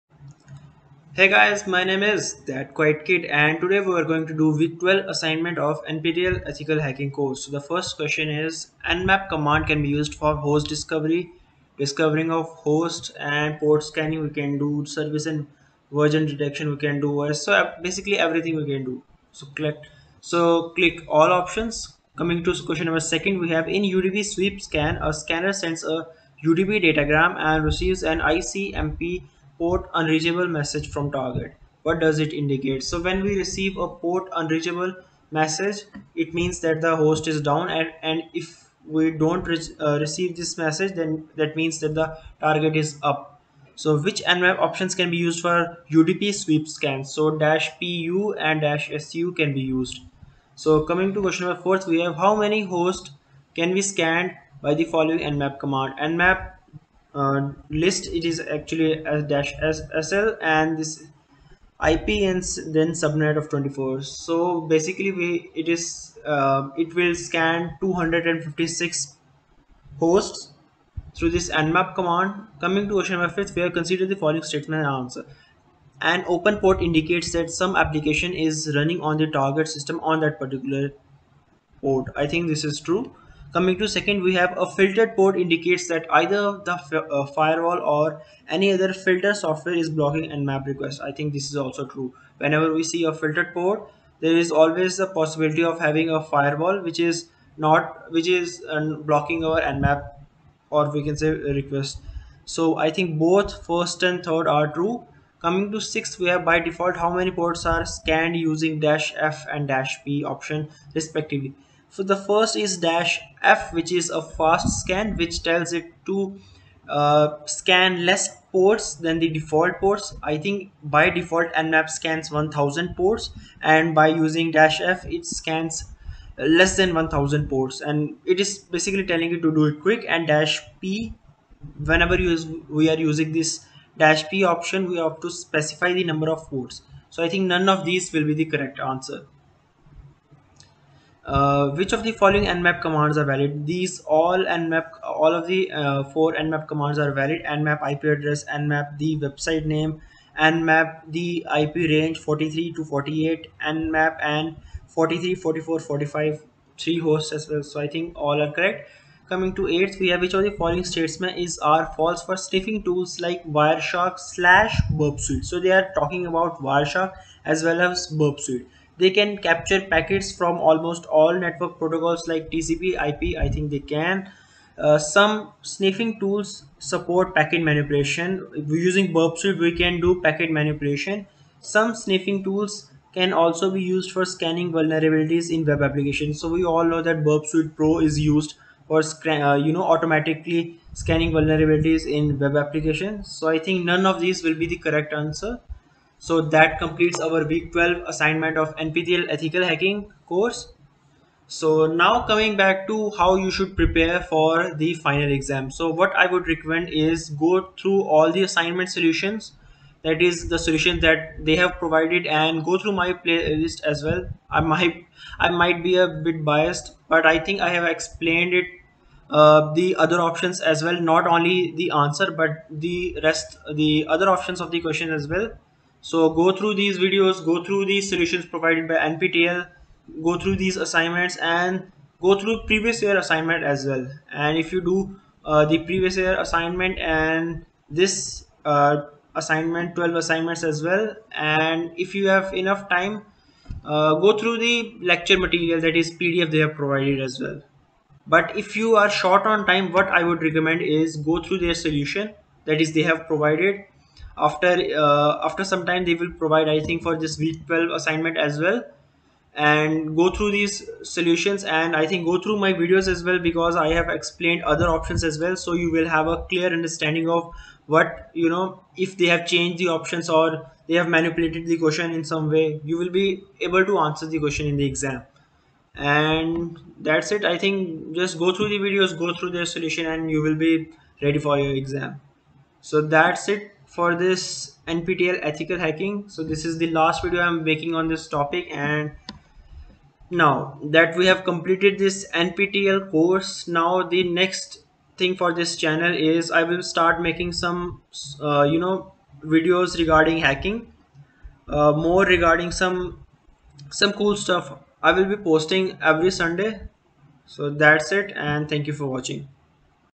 NPTEL Ethical Hacking Week 12 Assignment Answers
Answers:-
Q1:- a,b,c,d,e
Q2:- b
Q3:- b,c
Q4:- a
Q5:- c
Q6:- d
Q7:- a,b,c,d
Q8:- d

Timestamps:-
00:00 - Assignment Solution
03:21 - How you should prepare for the final exam
04:09 - If you don't have time(A day before exam)
04:50 - What's next on this channel